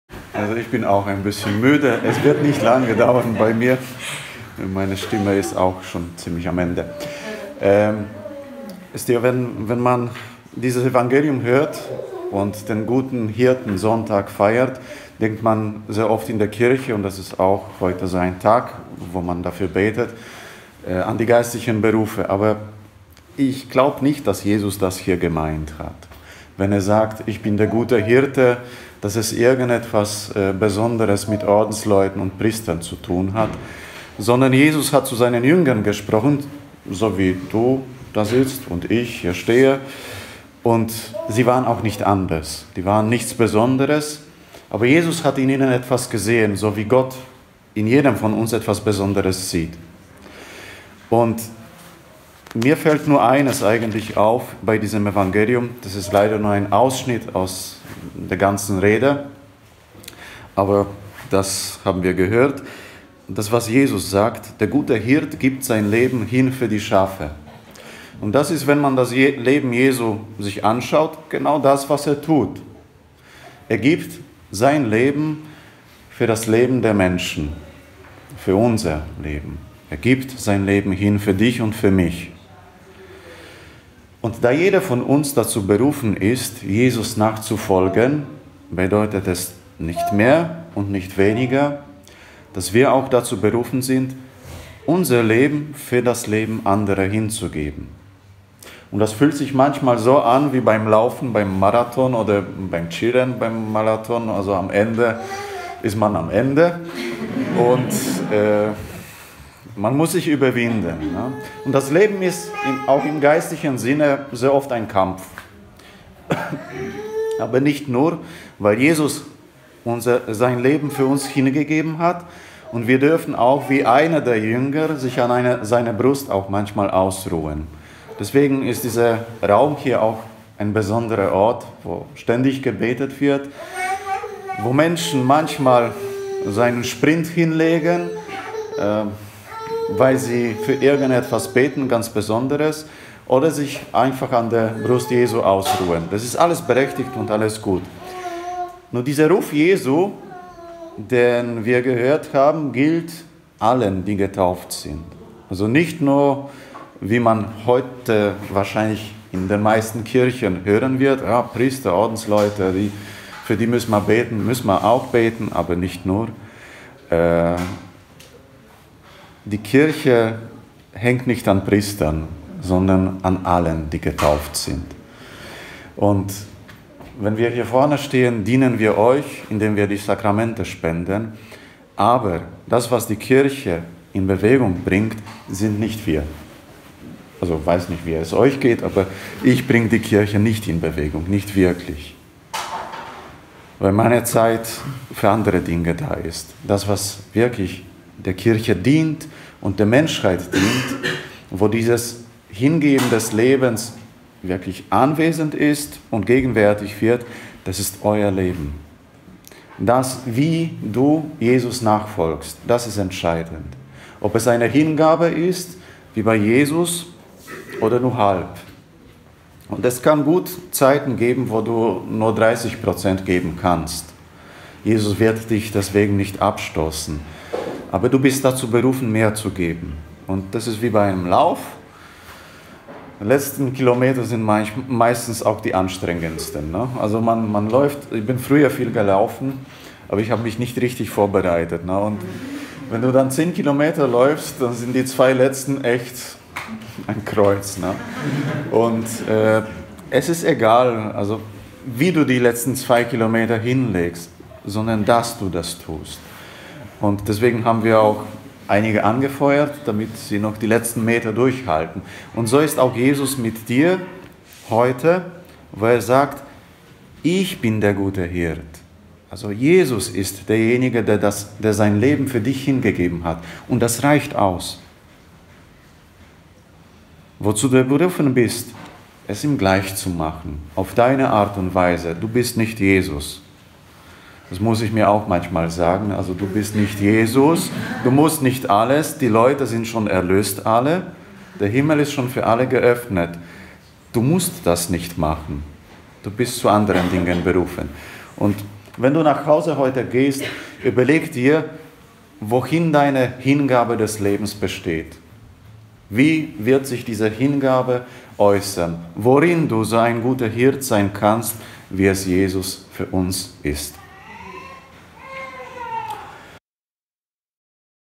4. Sonntag der Osterzeit B - Darius Lebok OFM (2018)
Eine Predigt zum 4. Sonntag der Osterzeit B von P. Darius Lebok OFM
zum Abschluss von Jog'n'pray bei Vienna City Marathon 2018

ERSTE LESUNG Apg 4, 8-12
PSALM Ps 118, 1 u. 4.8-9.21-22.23 u. 26.28-29
ZWEITE LESUNG 1 Joh 3, 1-2
EVANGELIUM Joh 10, 11-18